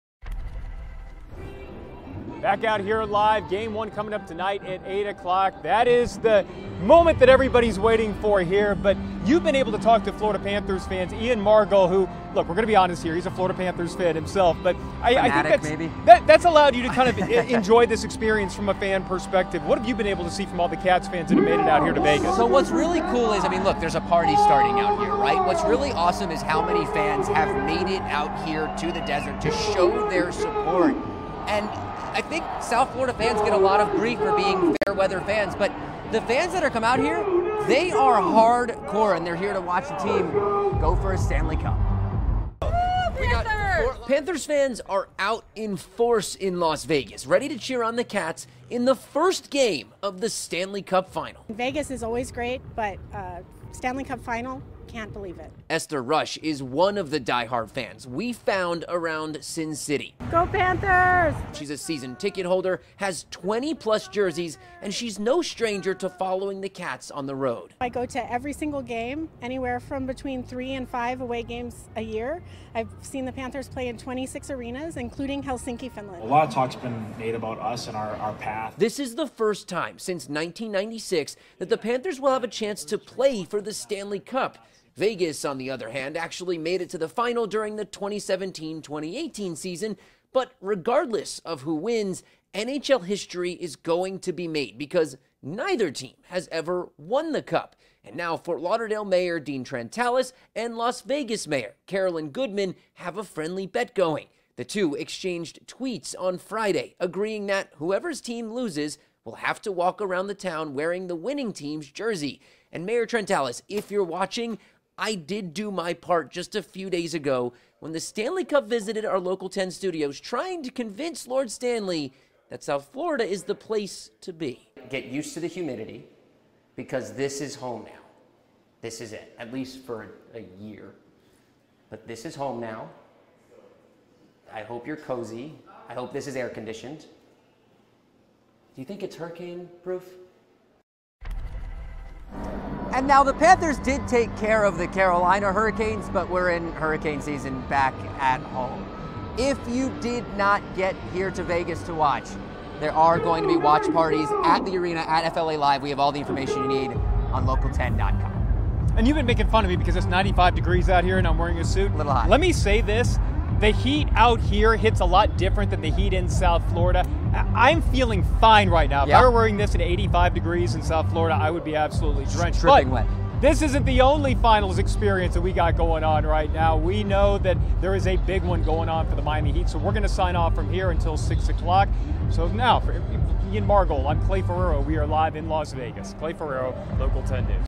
Florida Panthers fans ready to pack the T-Mobile Arena as Stanley Cup Final begins
Florida Panthers fans were out in full force in Las Vegas Saturday as the team gets ready to take on the Golden Knights in Game 1 of the Stanley Cup Final.